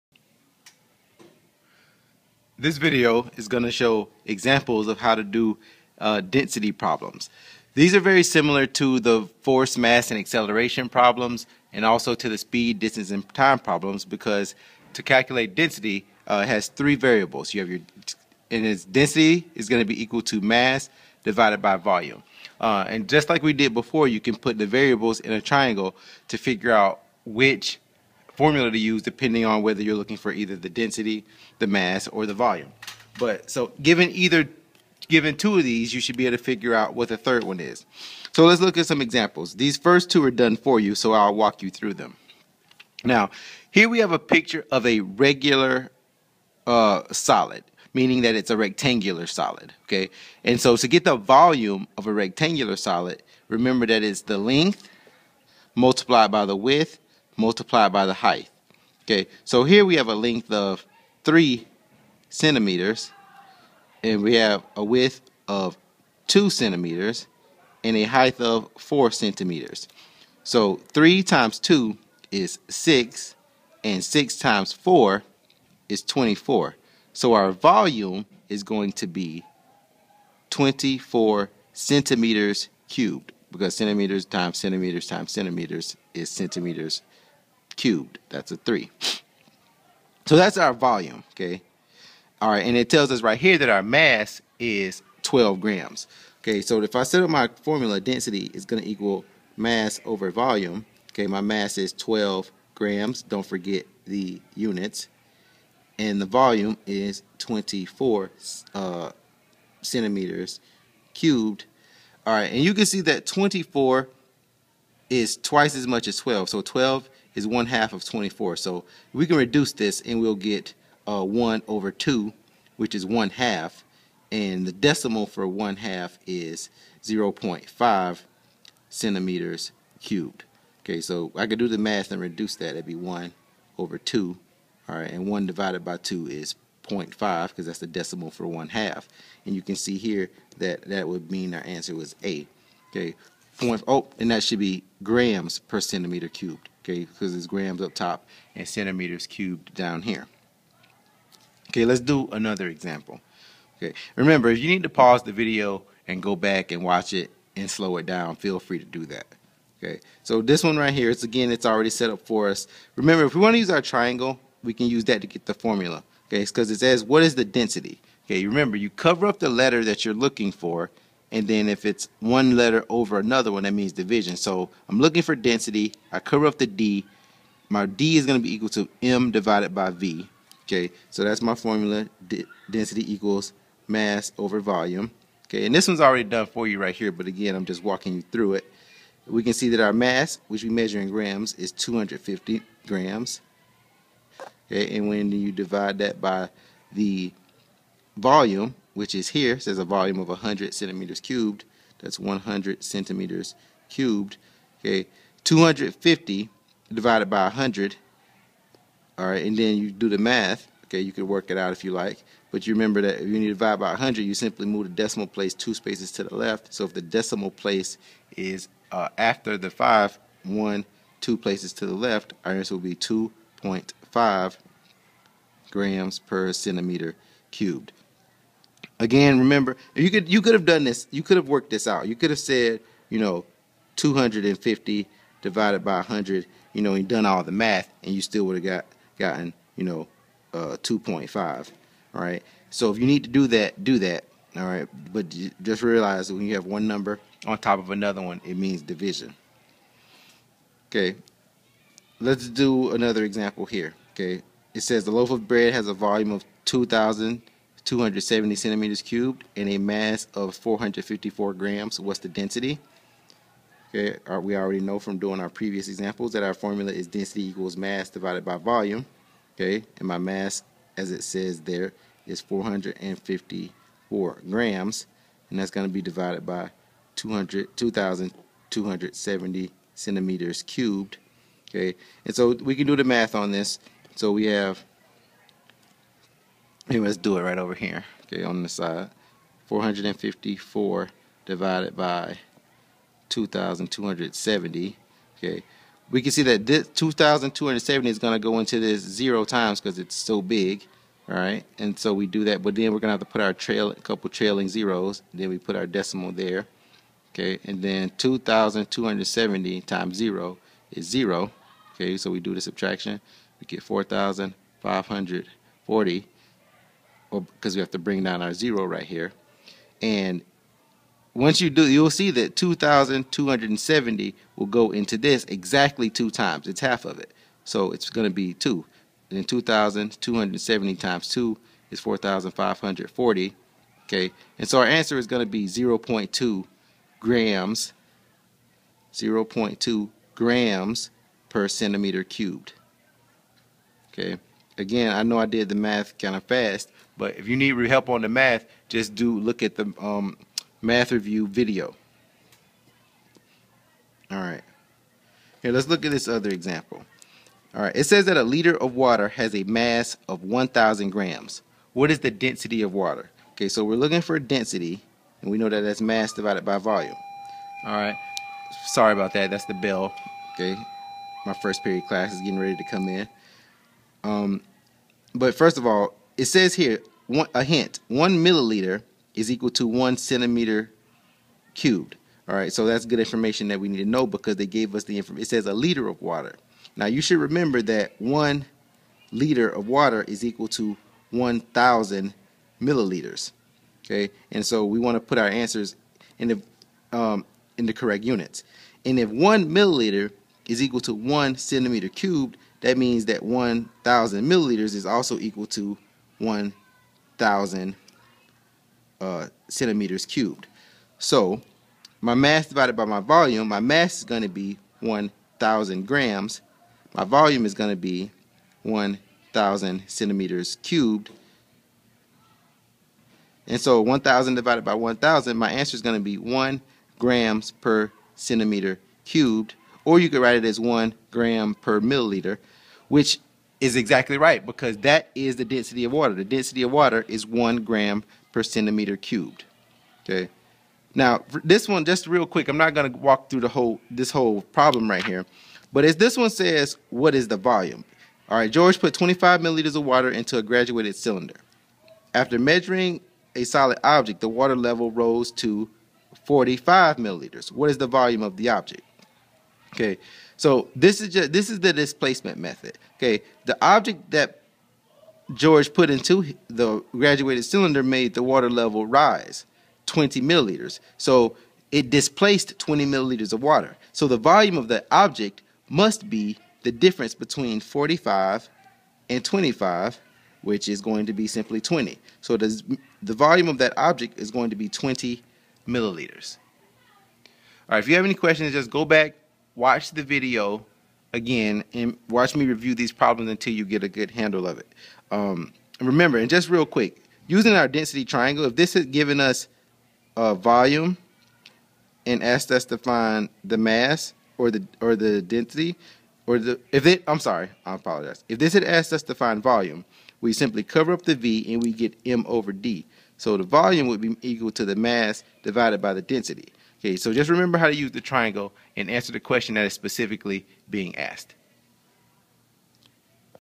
Examples of Density Problems
How to solve desire problems when given mass and volume.